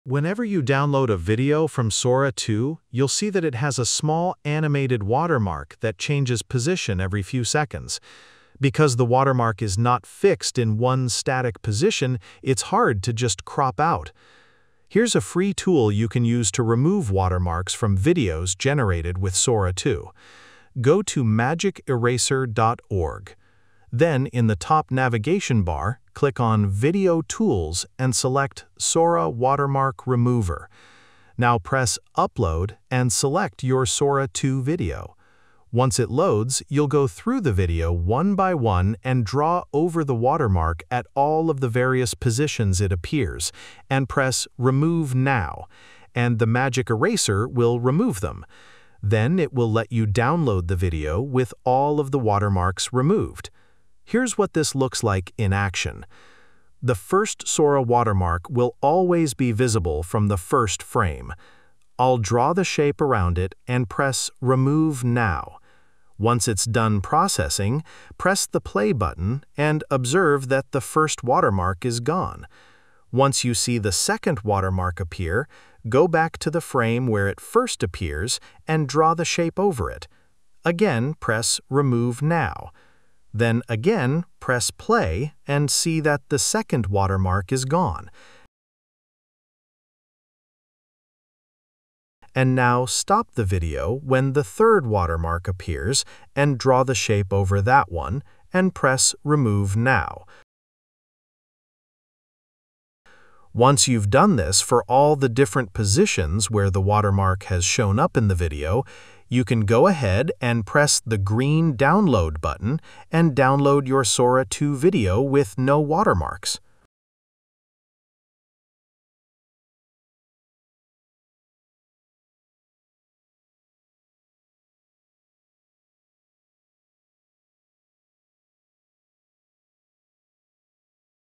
Sora 2 Watermark Removal Tutorial: FREE Tool to Remove Sora 2 Watermarks in Minutes (Step-by-Step)
Here’s the exact, step-by-step method to remove the moving Sora 2 watermark from your own generated videos using a free, browser-based tool—no paid plugins or complex editing required.

If you’ve noticed that Sora 2 embeds a small animated watermark that keeps shifting position, simple cropping won’t cut it. In this tutorial, I show how to cleanly erase each occurrence so your own AI-generated footage is presentation-ready.

Why Sora 2’s moving watermark is hard to crop out

How to use MagicEraser→ Video Tools → “Sora Watermark Remover”

The fastest workflow to paint over each watermark position and remove it

How to preview your results and export a clean video

Step-by-step (quick recap)

Go to MagicEraser website → Video Tools → Sora Watermark Remover

Upload your Sora 2 video

Scrub to the frame where the watermark appears, draw over it, click Remove Now

When the watermark moves, jump back to the first frame where the new position appears, draw again, Remove Now

Repeat for all positions → Download the final video

Chapters

00:00 Intro: Why Sora 2’s watermark keeps moving
00:31 Open MagicEraser & upload your Sora video
00:56 Remove the first watermark instance
01:20 Remove the next watermark position
01:32 Clean up remaining positions
01:56 Export & download your watermark-free edit

Pro tips

Work frame-accurately: jump to the first frame where a new watermark position appears before removing.

After each removal, hit Play to confirm it’s gone before moving on.

If you see faint artifacts, zoom in and redraw tighter around the logo, then remove again.

This tutorial is intended only for removing watermarks from your own Sora-generated videos where you hold the rights or have explicit permission.

Tools mentioned

MagicEraser → Video Tools → Sora Watermark Remover